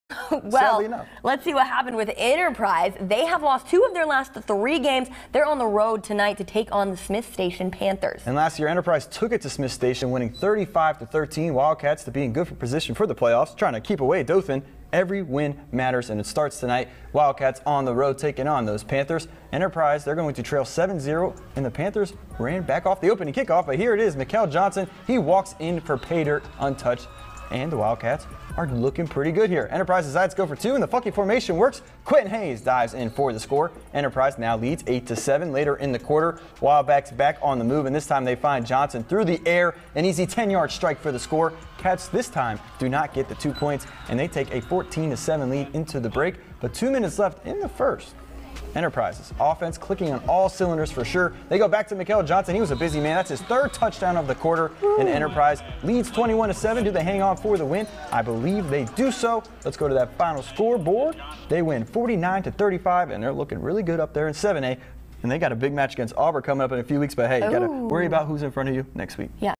The Extra Point: Enterprise vs Smiths Station
The Enterprise Wildcats marched up the state and beat Smiths Station, 49-35!